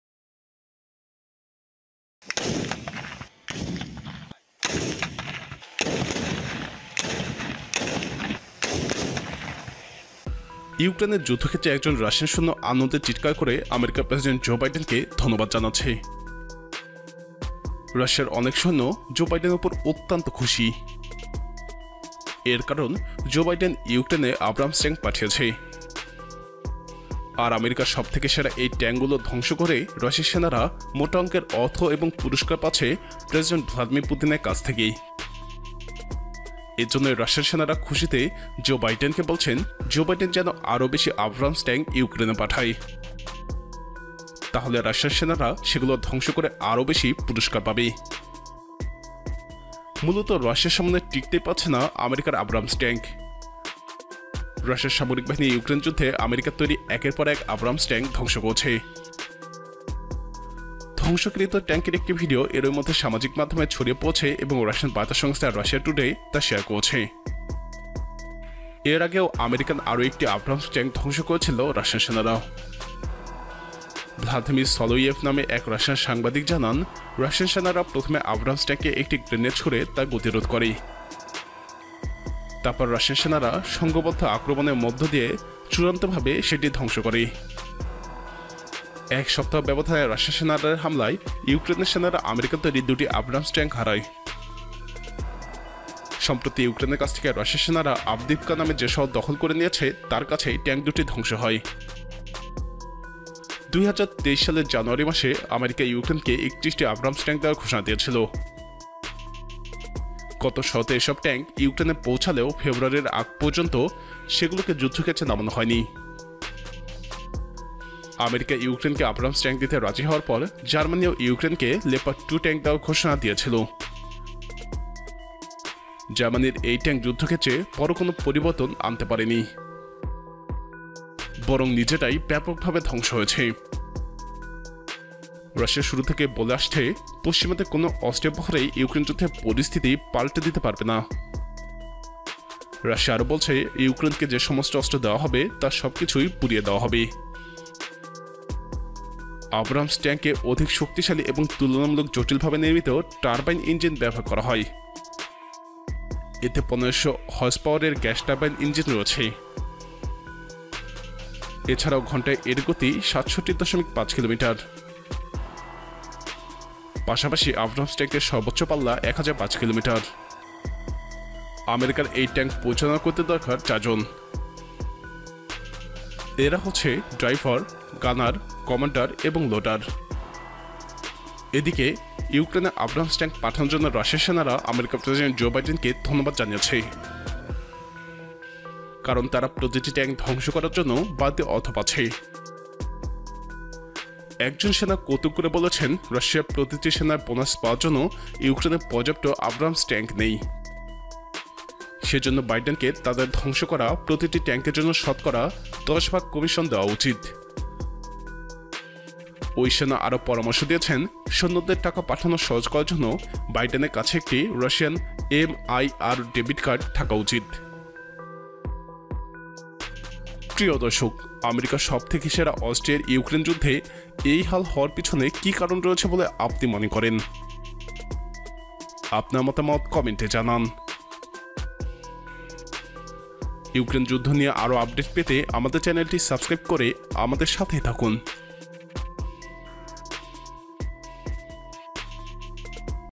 রাশিয়ার সামনে দাড়াতেই পারছে না আমেরিকা। ইউক্রেনে আমেরিকার গর্বকে পুড়িয়ে দিচ্ছে রাশিয়া। টেক দুনিয়া
এর কারন জো বাইডেন ইউক্রেনে আব্রামস ট্যাংক পাঠিয়েছে৷ আর আমেরিকার সবথেকে সেরা এই ট্যাংকগুলো ধ্বংস করে রাশিয়া সেনারা মোটাংকের অর্থ এবং পুরল্কার পাচ্ছে প্রেসিডেন্ট পুতিনের কাছে থেকে৷ এজন্যই রাশিয়ান সেনারা খুশিতে জো বাইডেনকে বলছেন, জো বাইডেন যেন আরও বেশি আব্রামস ট্যাংক ইউক্রেন পাঠায়৷ তাহলে রাশিয়ার সেনারা তা ধ্বংস করে আরও বেশি পুরস্কার পাবে৷ মুলত রাশিয়ার সামনে টিকতে পারছে না আমেরিকার আব্রামস ট্যাংক৷ রাশিয়ার সামরিক বাহিনী ইউক্রেন যুদ্ধে মার্কিন নির্মিত একের পর এক আব্রামস ট্যাঙ্ক ধ্বংস করেছে। ধ্বংসকৃত ট্যাংকের একটি ভিডিও এরইমধ্যে সামাজিক মাধ্যমে ছড়িয়ে পড়েছে এবং রুশ বার্তা সংস্থা রাশিয়া টুডে তা শেয়ার করেছে।